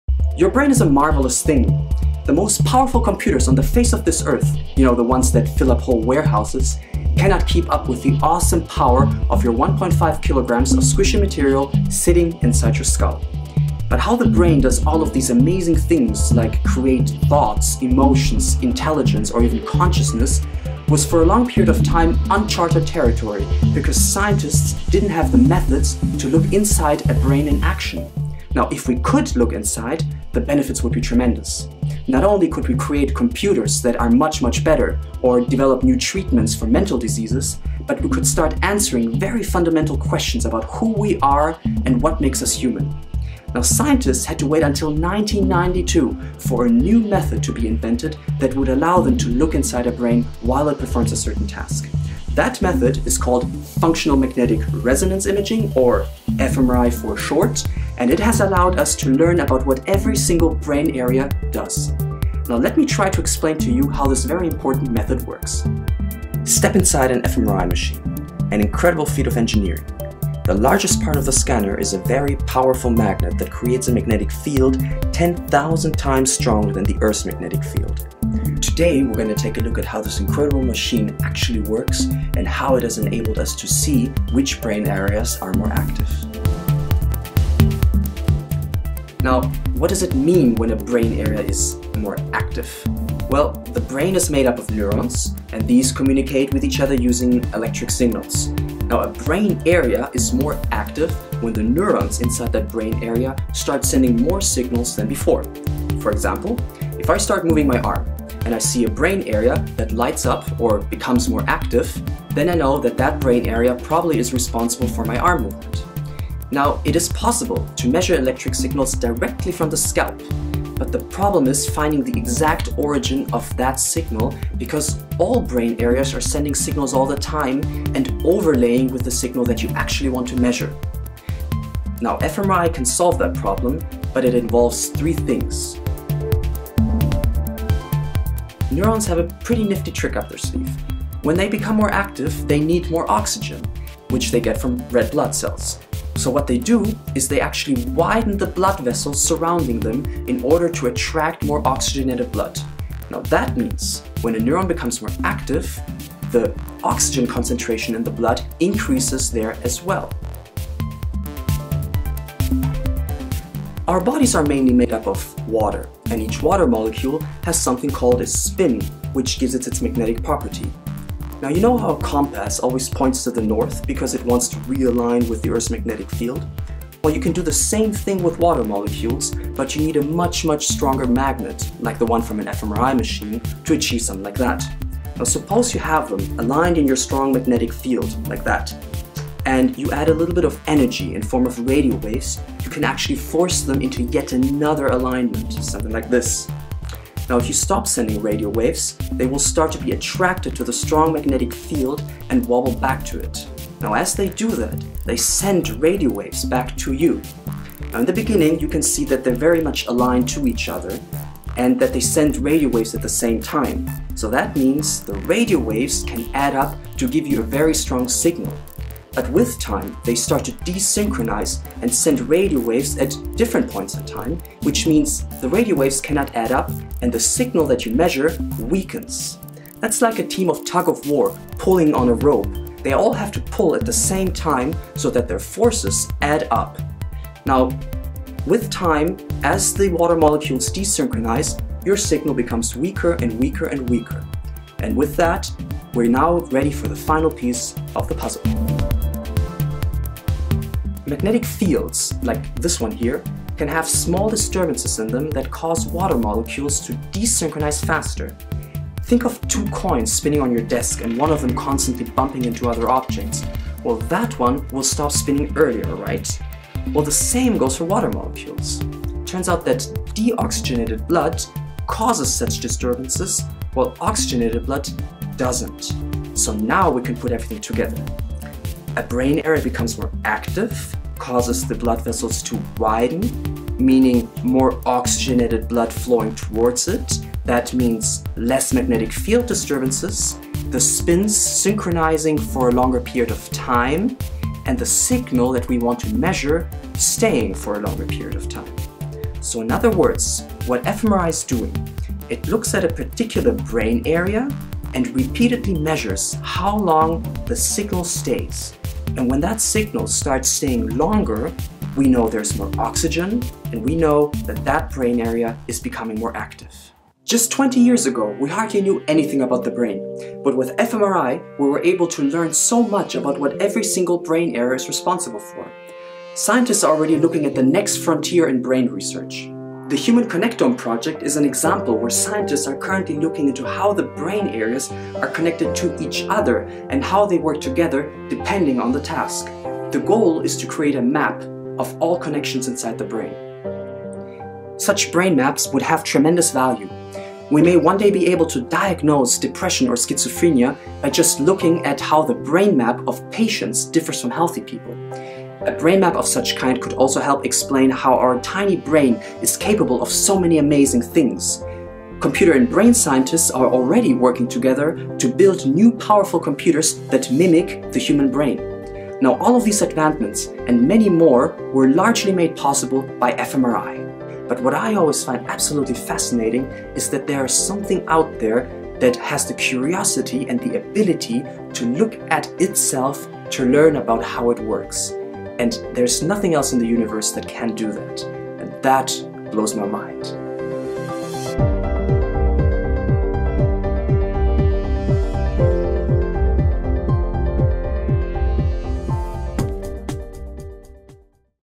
fMRI - How it Works and What it's Good For
ENGLISH
For a long period of time we didn't know anything about that brain. That's because it's extremely hard to measure changes in electric signals that indicate brain activity.

Scientists had to wait until 1992 for the invention of functional Magnetic Resonance Imaging (fMRI) in order to observe brain activity.

This video was made for the competition, but is and will remain freely available for all interested in learning a bit more about how brain scientists use for their work.

DEUTSCH
Das Gehirn war für eine sehr lange Zeit ein Mysterium. Das liegt daran, dass die elektrischen Ströme, die Aufschlüsse über die Funktionsweise des Gehirns liefern können, extreme schwach und schwer zu messen sind.
Wissenschaftler mussten sich bis 1992 gedulden. Die funktionale Magnetresonanztomographie (fMRT) ermöglicht den Blick in das aktive Gehirn.

Dieses Video erklärt, wie fMRT funktioniert und zu welchem Wissen über das Gehirn es uns verholfen hat. fMRT bleibt ist auch heute noch ein Platzhirsch unter den Methoden der Neurowissenschaftler und wird auch in Zukunft vieles über das Gehirn in Erfahrung bringen.

Dieses Video wurde für den Wettbewerb produziert, bleibt aber weiterhin für alle zugänglich, die Interesse daran haben zu lernen, was die Hirnforschung so alles macht.